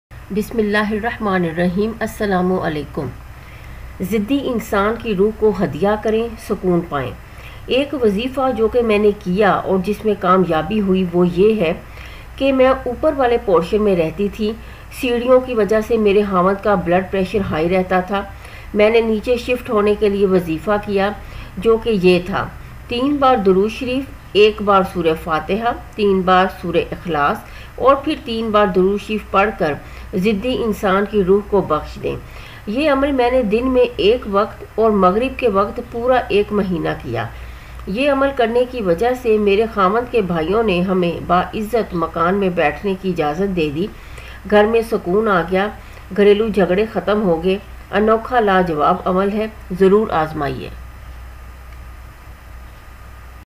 Wazifa For Rude Person | Ubqari Wazaif
In this video wazifa is explained  to change the behavior of obstinate person.